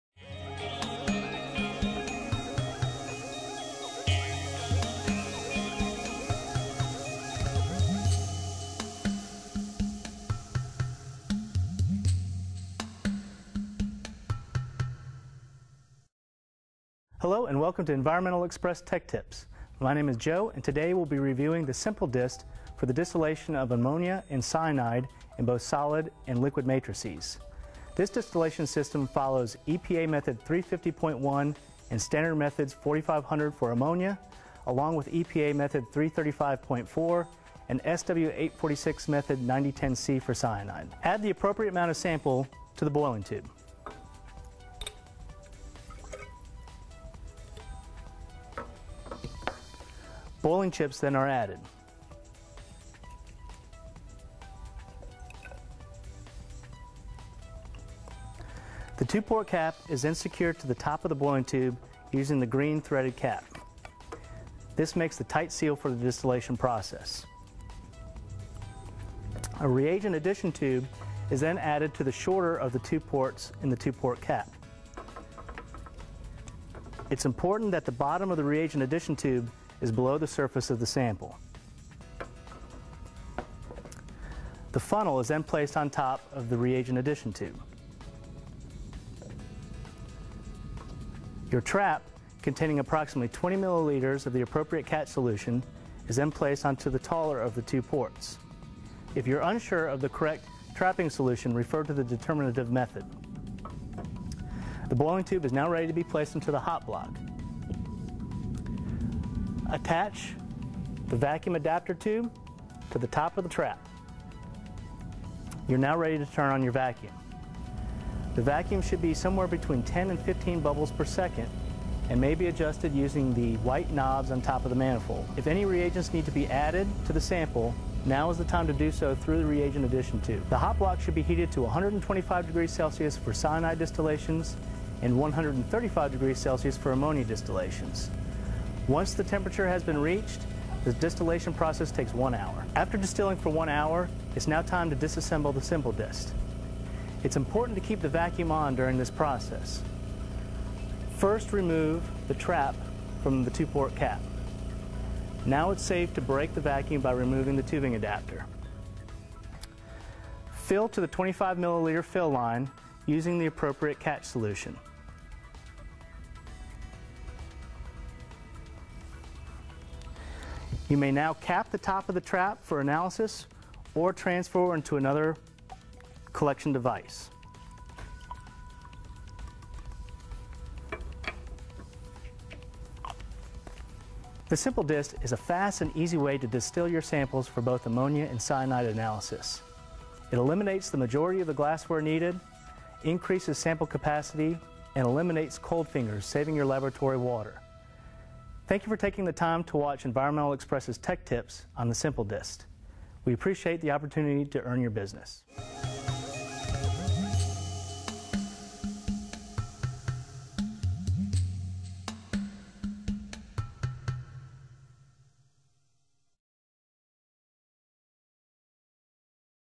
SimpleDist for Ammonia and Cyanide Distillations
The optimal system for performing cyanide, phenol, and ammonia distillations following established  EPA methods 335.4 and 350.1, and Standard Methods 4500-CN-C and 4500-NH3.